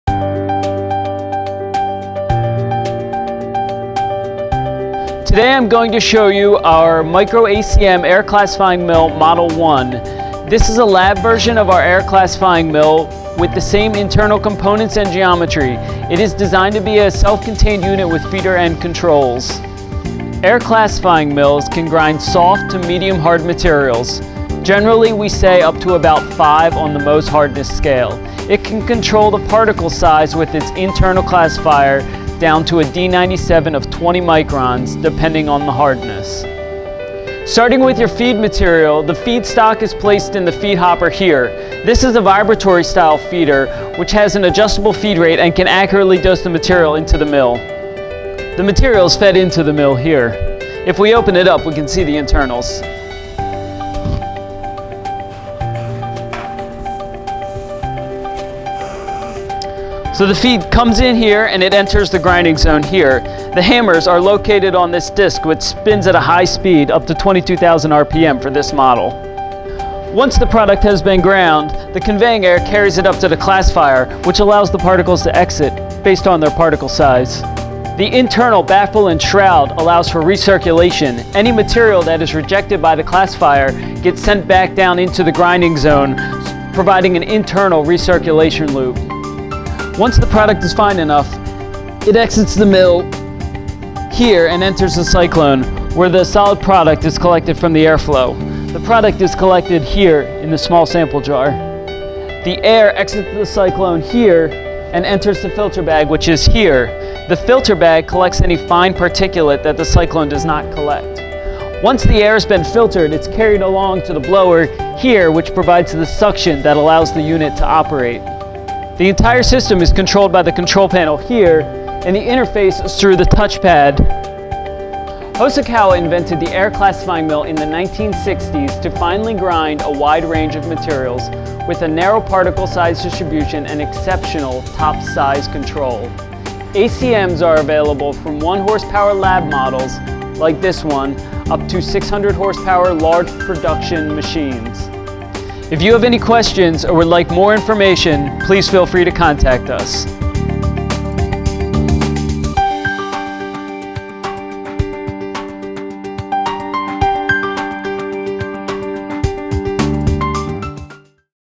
Lab Scale Air Classifying Mill
The ACM 1 is one of Hosokawa's smallest air classifying mills available. It was designed to process small batches of research materials down to D 97  = 20 microns. This video quickly tours this self contained laboratory milling system.